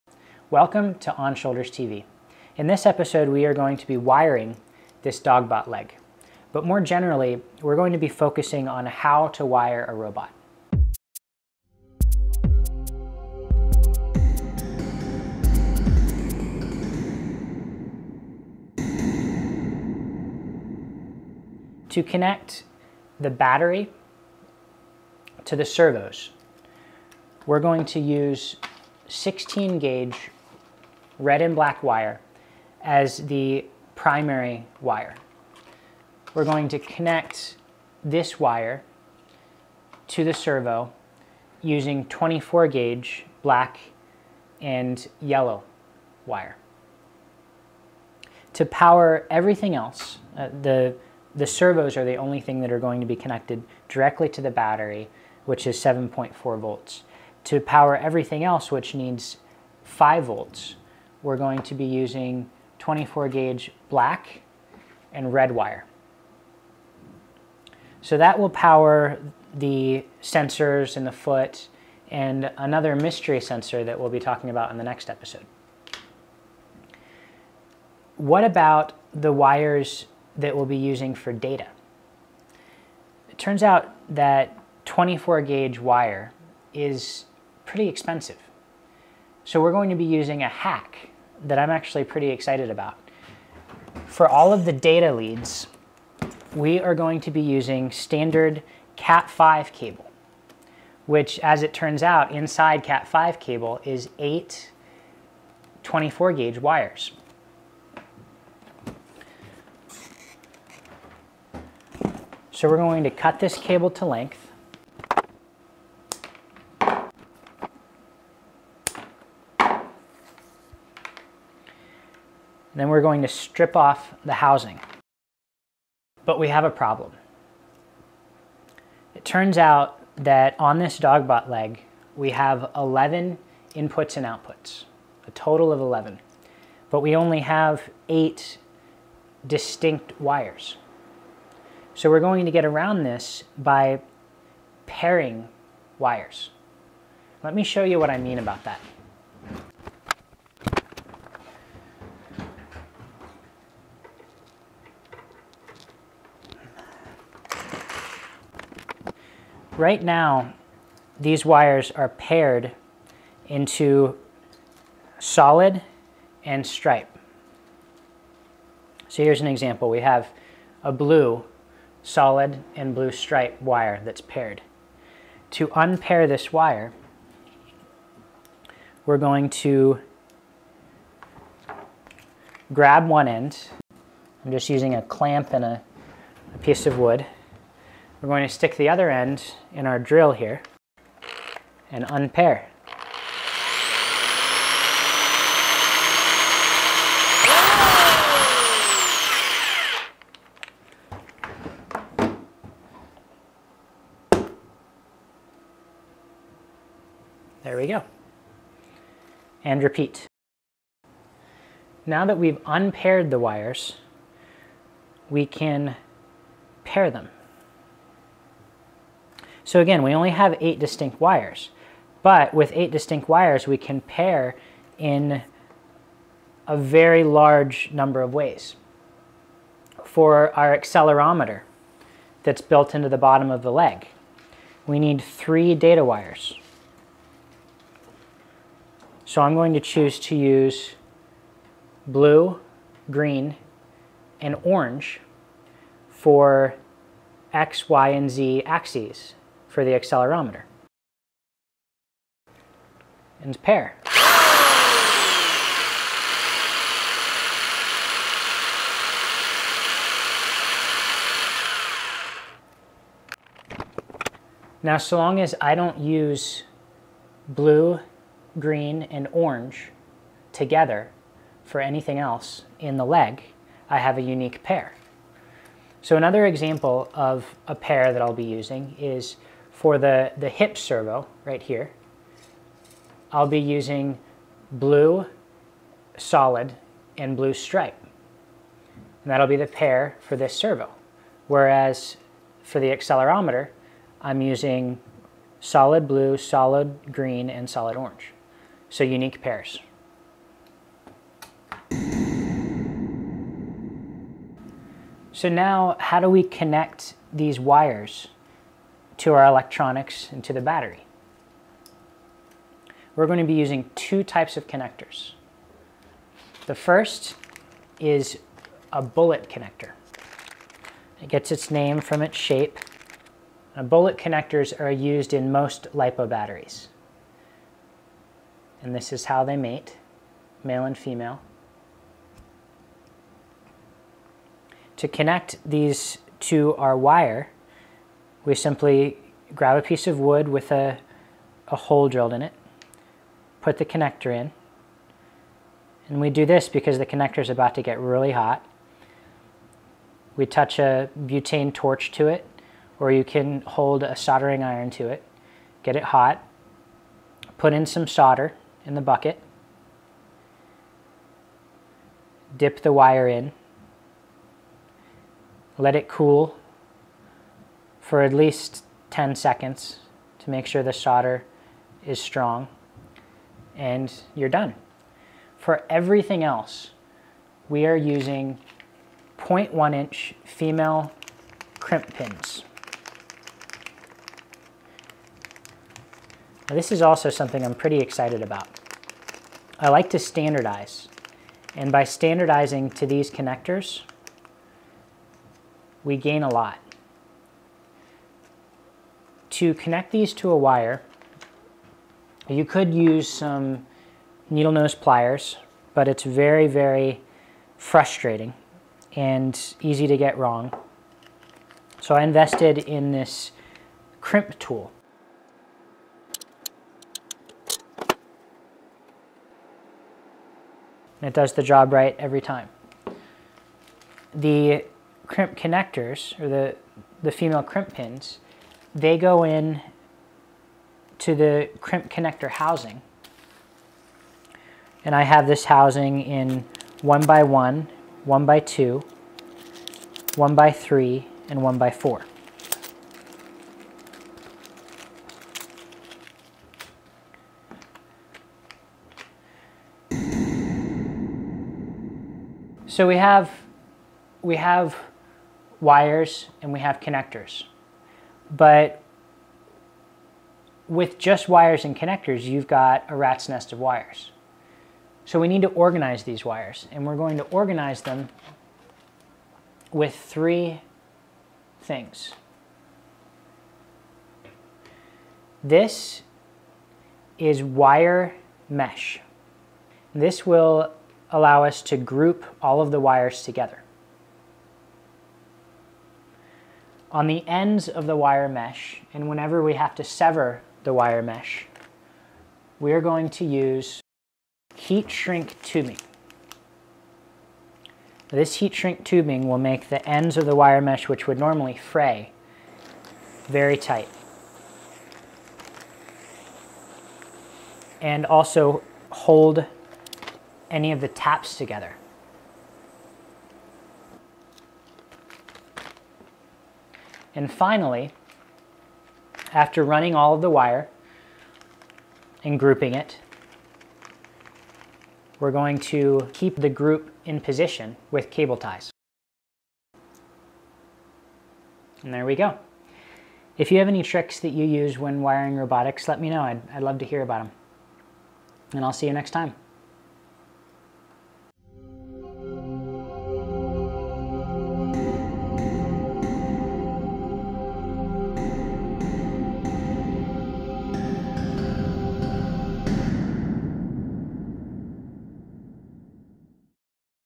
onshouldersTv: How to Wire a Robot
In this episode we consider what goes into wiring a robot. We look at a cheap source of 24 gauge wire, look at standardized connectors, and go over wire organization. - Season 3 : Ep.